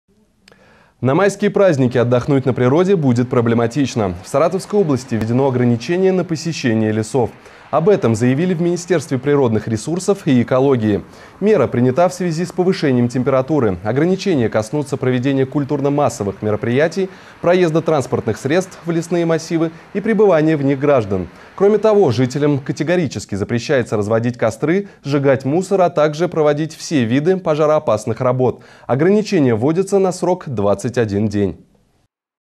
На майские праздники отдохнуть на природе будет проблематично
В Саратовской области введены ограничения на посещение лесов.Об этом заявили в министерстве природных ресурсов и экологии. Материал вышел в эфир 29 апреля 2015 года.